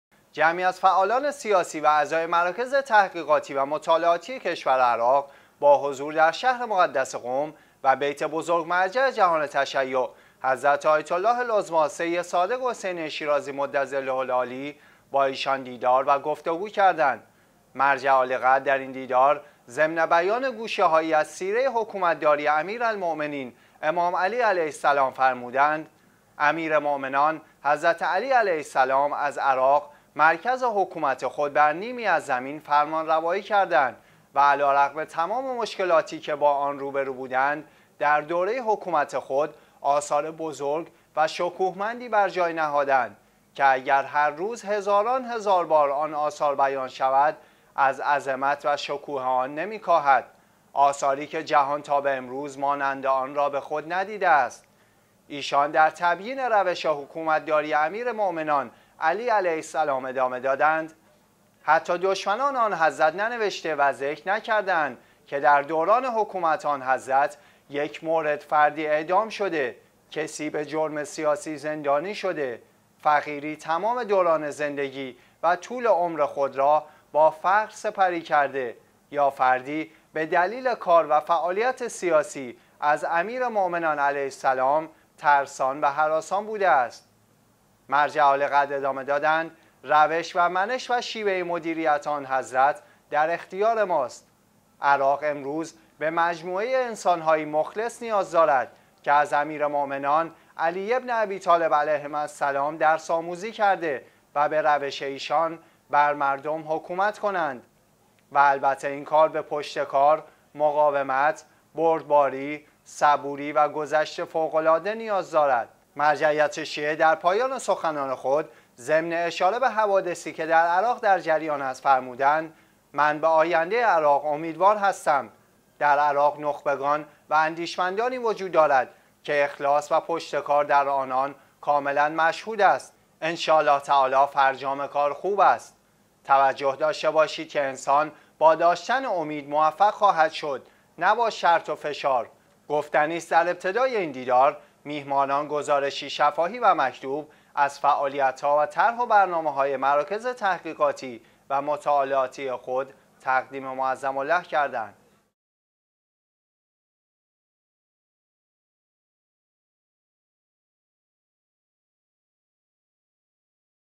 حضرت آیت الله العظمی شیرازی: عراق از نخبگان و مخلصان و افراد دارای پشتکار....
جمعی از فعالان سیاسی و اعضای مراکز تحقیقاتی و مطالعاتی کشور عراق با حضور در شهر مقدس قم و بیت بزرگ مرجع جهان تشیع حضرت آیت الله العظمی سید صادق حسینی شیرازی مدظله العالی با ایشان دیدار و گفتگو کردند.
مرجع عالیقدر در این دیدار ضمن بیان گوشه هایی از سیره حکومت داری امیرالمومنین امام علی علیه السلام فرمودند: امیرمؤمنان حضرت علی علیه السلام از عراق، مرکز حکومت خود بر نیمی از زمین، فرمانروایی کردند و علی رغم تمام مشکلاتی که با آن روبرو بودند، در دوره حکومت خود آثار بزرگ و شکوهمندی بر جای نهادند که اگر هر روز هزاران هزار بار آن آثار بیان شود، از عظمت و شکوه آن نمی کاهد؛ آثاری که جهان تا به امروز مانند آن را به خود ندیده است.
ایشان در تبیین روش حکومت داری امیرمومنان على عليه السلام ادامه دادند: حتی دشمنان آن حضرت ننوشته و ذکر نکرده اند که در دوران حکومت آن حضرت یک مورد فردی اعدام شده، کسی به جرم سیاسی زندانی شده، فقیری تمام دوران زندگی و طول عمر خود را با فقر سپری کرده یا فردی به دلیل کار و فعالیت سیاسی از امیرمؤمنان علیه السلام ترسان و هراسان بوده است.
مرجع عالیقدر ادامه دادند: روش و منش و شیوه مدیریت آن حضرت در اختیار ما است. عراق امروز، به مجموعه انسان هایی مخلص نیاز دارد که از امیرمومنان علی بن ابی طالب علیهما السلام درس آموزی کرده و به روش ایشان بر مردم حکومت کنند و البته این کار به پشتکار، مقاومت، بردباری، صبوری و گذشتِ فوق العاده نیاز دارد.
مرجعیت شیعه در پایان سخنان خود ضمن اشاره به حوادثی که در عراق در جریان است فرمودند: من به آینده عراق امیدوار هستم. در عراق نخبگان و اندیشمندانی وجود دارد که اخلاص و پشتکار در آنان کاملاً مشهود است. ان شاء الله تعالی فرجام کار خوب است. توجه داشته باشید که انسان با داشتن امید موفق خواهد شد، نه با شرط و فشار.
گفتنی است در ابتدای این دیدار میهمانان گزارشی شفاهی و مکتوب از فعالیت ها و طرح و برنامه های مراکز تحقیقاتی و مطالعاتی خود تقدیم معظم له کردند.